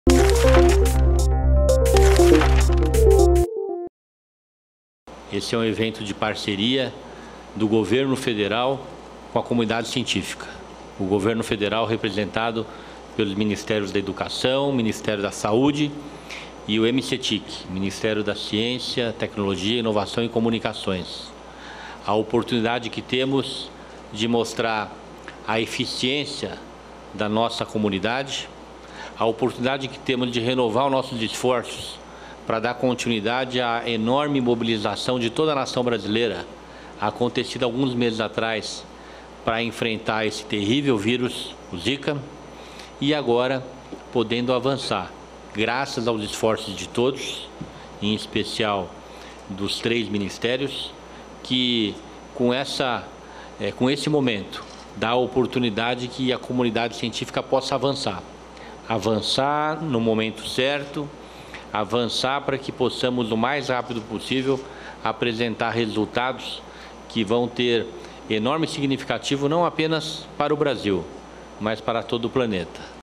Kassab destaca que investimento é oportunidade para pesquisa contra o zika
EM DESTAQUE - 02.06.16: Em ação integrada, governo federal disponibiliza R$ 65 milhões para pesquisas sobre zika e controle do Aedes Aegypti. O ministro da Ciência, Tecnologia, Inovações e Comunicações, Gilberto Kassab, destaca que investimento é uma oportunidade para que a comunidade científica possa avançar e apresentar resultados significativos.